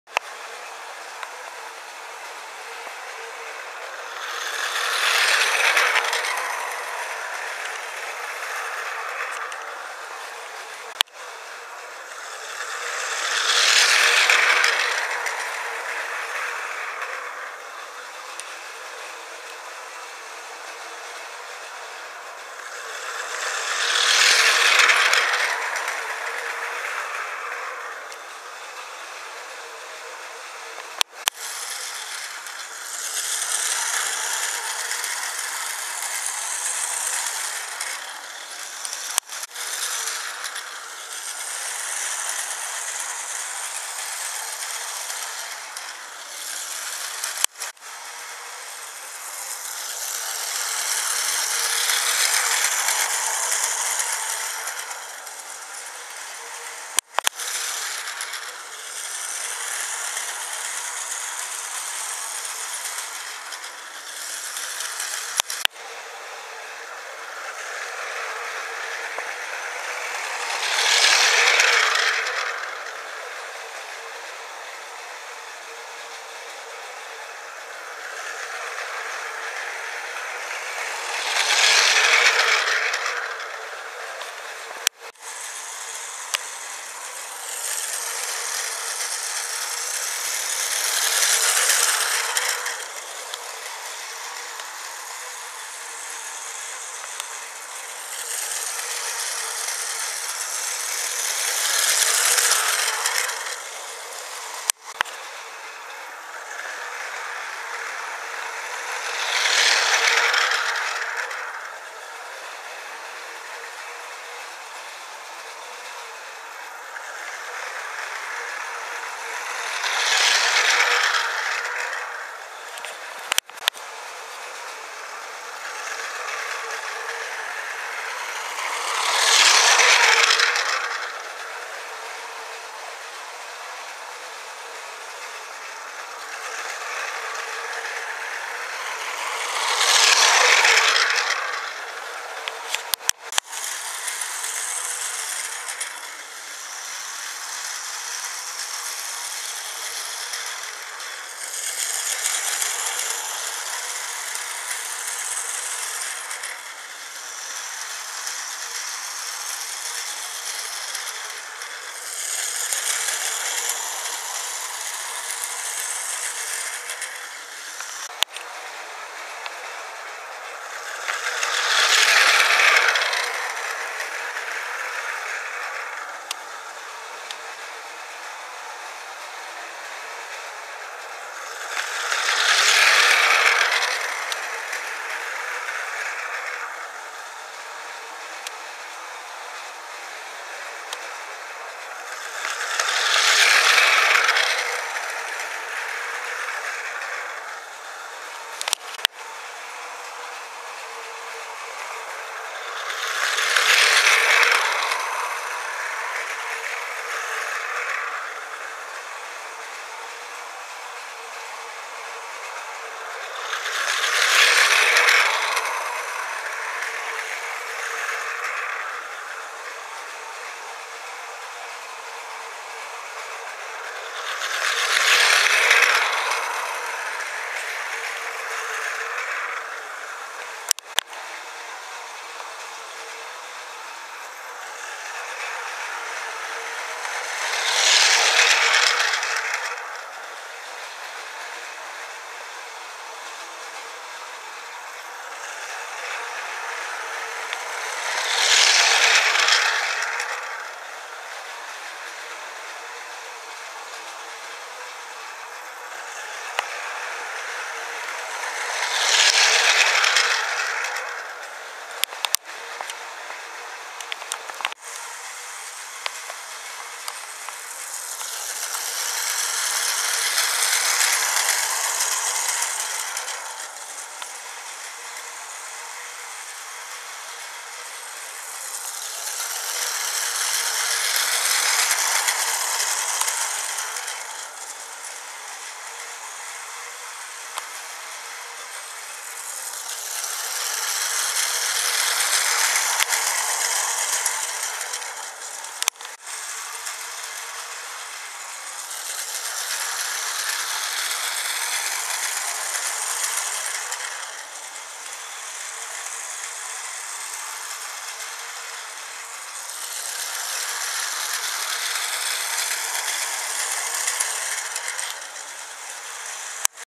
Video for Children Toy Trains Tomy White Bullet Train Set for Kiddies
Ho hey guys, some more shots of the battery operated Plarail HST running thru my Eurotunnel set.  :D    Not the Eurostar but the high speed train.
(Copyright vxbatabs 2014)